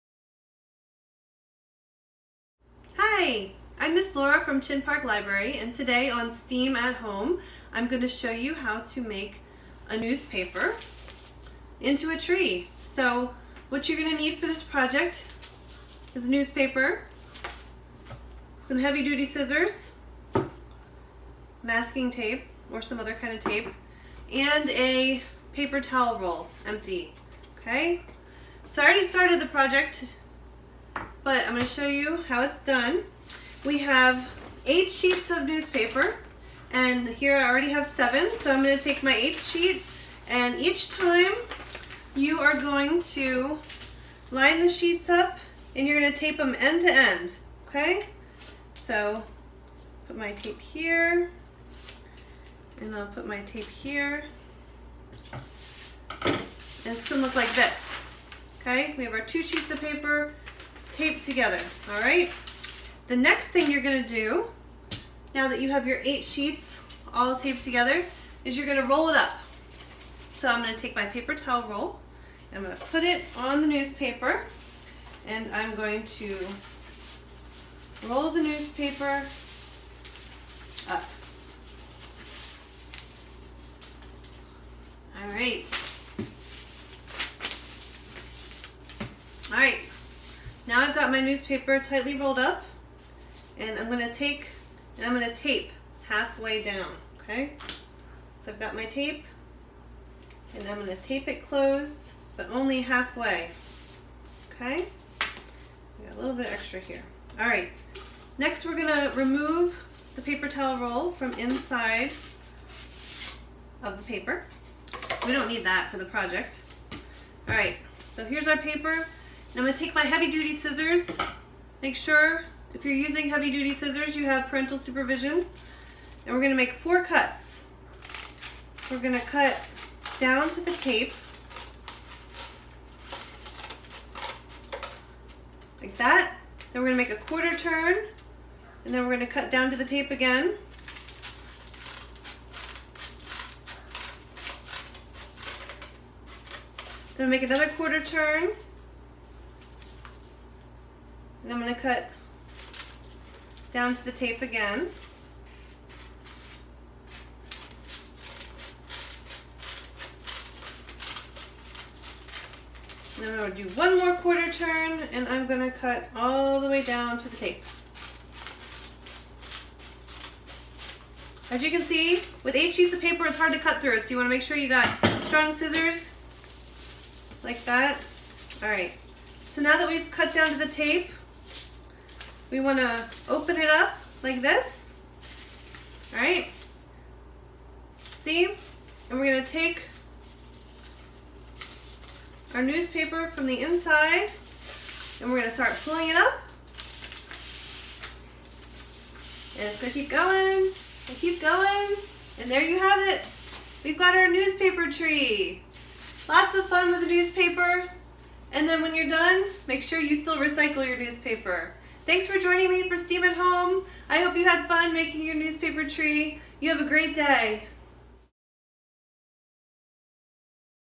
STEAM @ Home: Newspaper Tree
Join Ms. Laura from Chinn Park Regional Library for today's STEAM @ Home! She'll teach us how to make a Newspaper Tree.